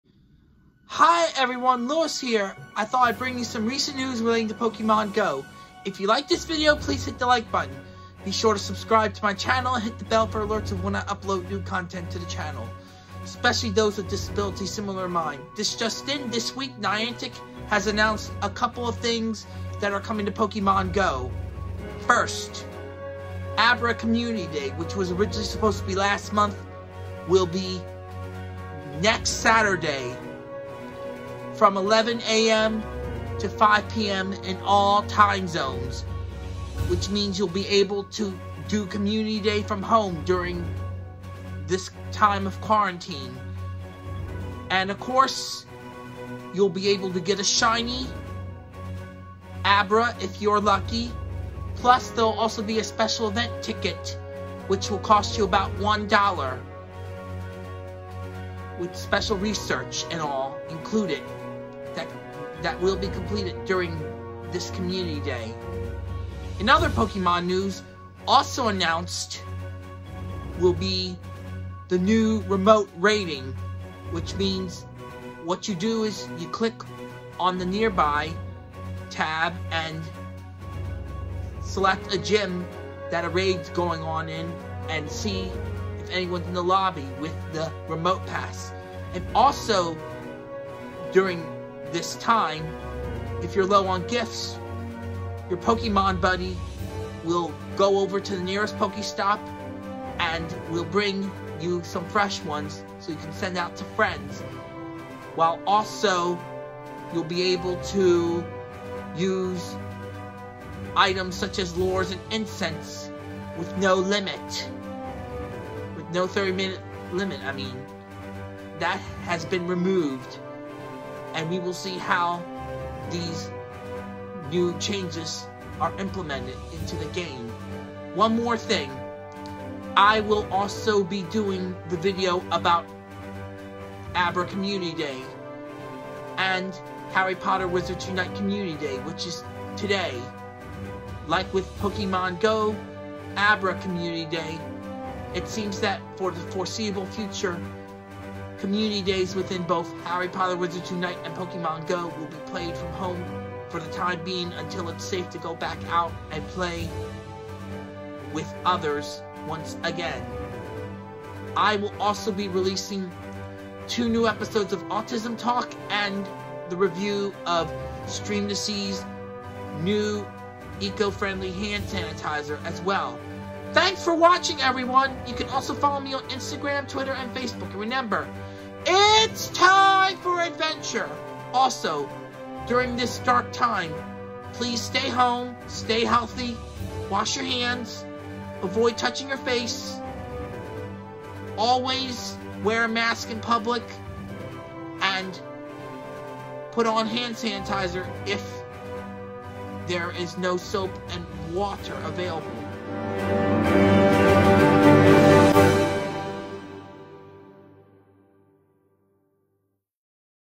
Pokémon Go News Episode 147 (Abra Community Day, Remote Raids)
In this video I talk about the rescheduled Abra Community Day from home and the remote raids due to the Coronavirus crisis.